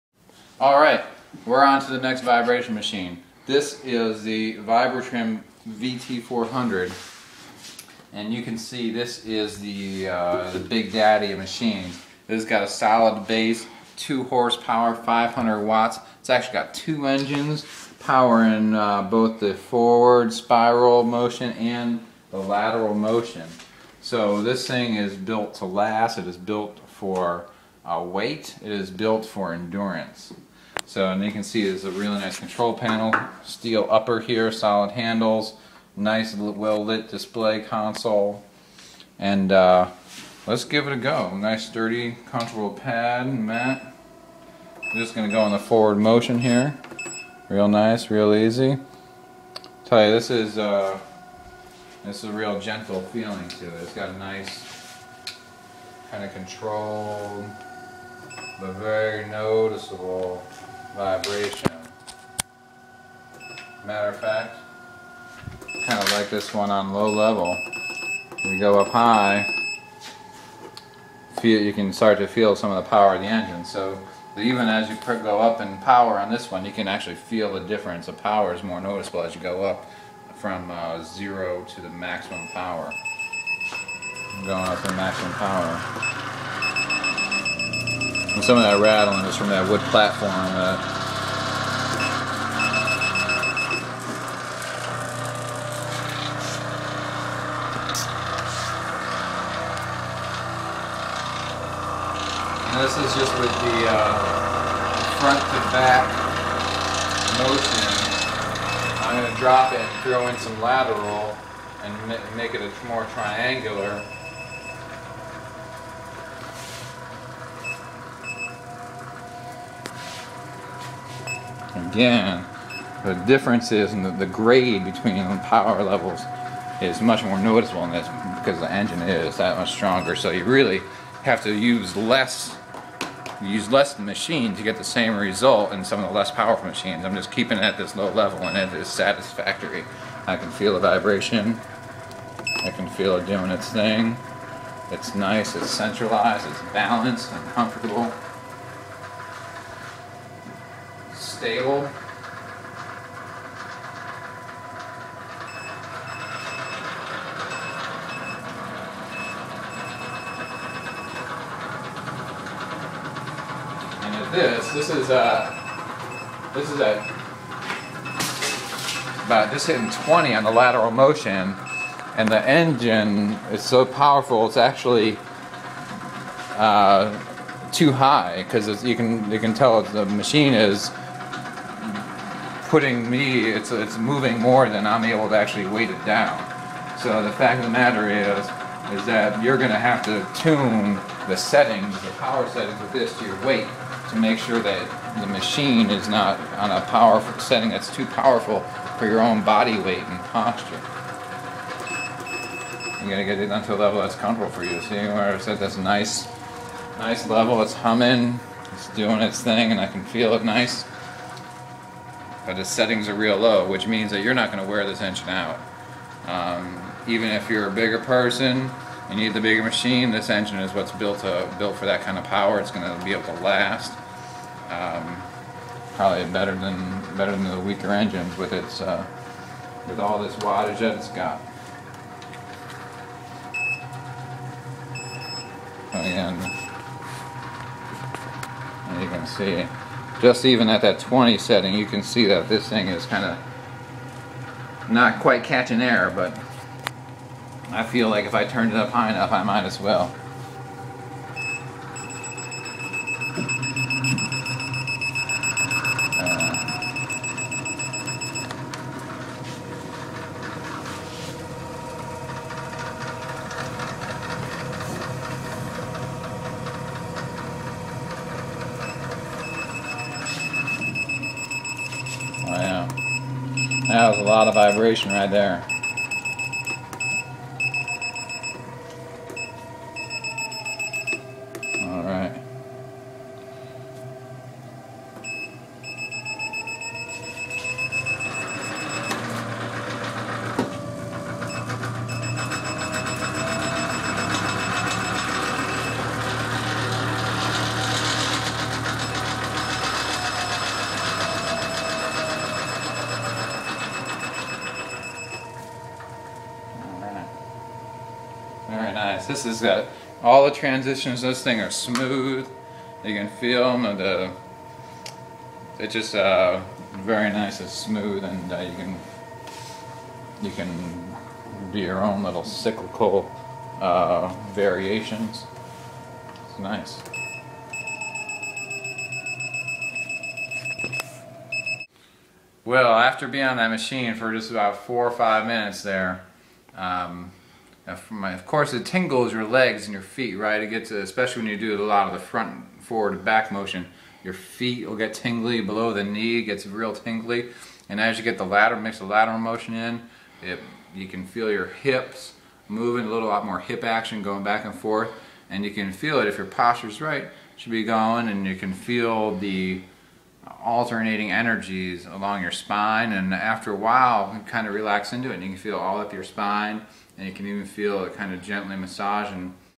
vt400
Testing out the Vibra-Trim VT400 Spiral